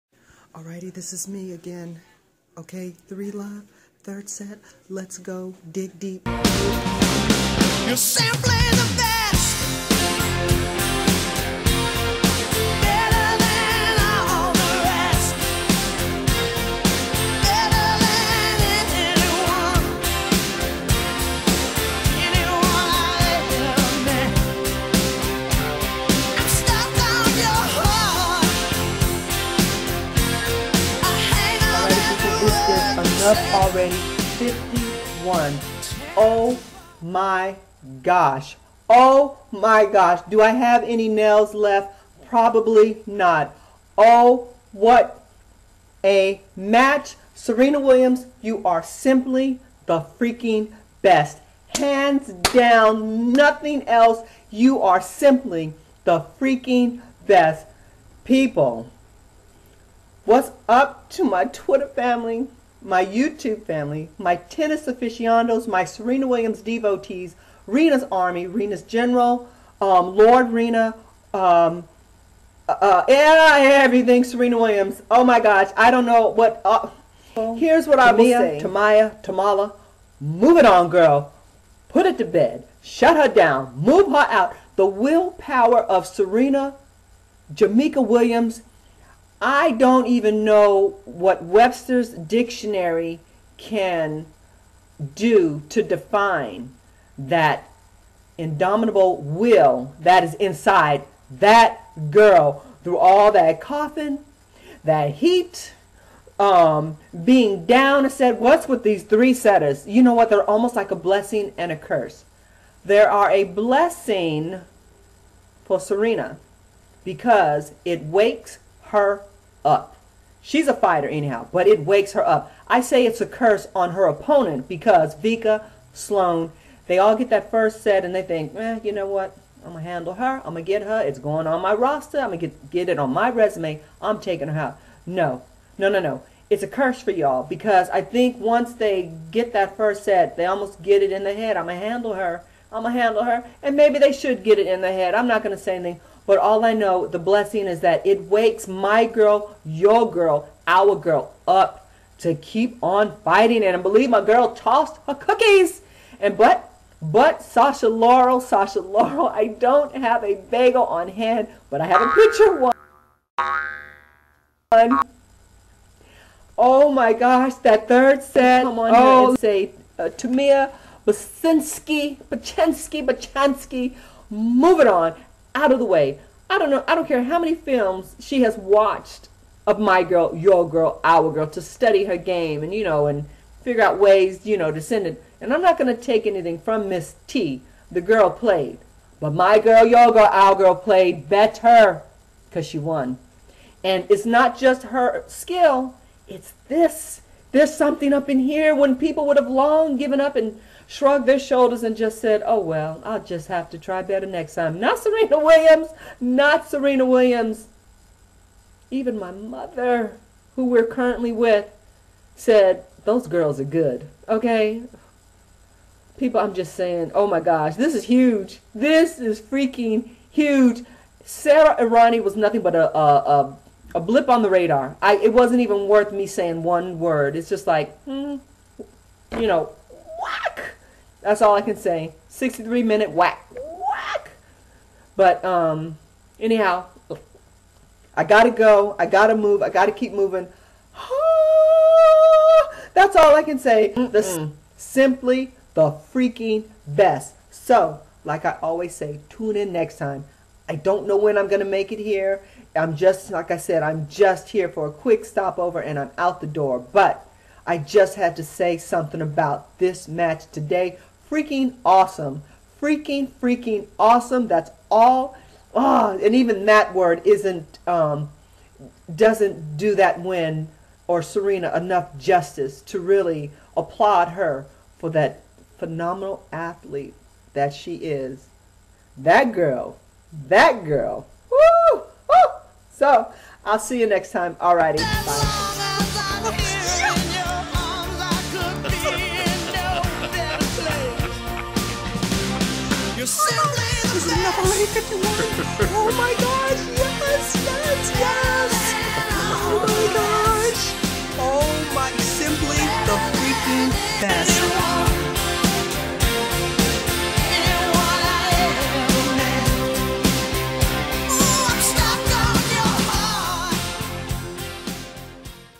SERENA WILLIAMS FRENCH OPEN 2015 SIMPLY THE BEST!
SERENA WILLIAMS is hands down the freakin' BEST! Today's win against Timea Bacsinszky (4-6, 6-3, 6-0) - AMAZING! What a fighter! On to the FINALS! Let's do THIS! #20! Ahhhhhhhhh!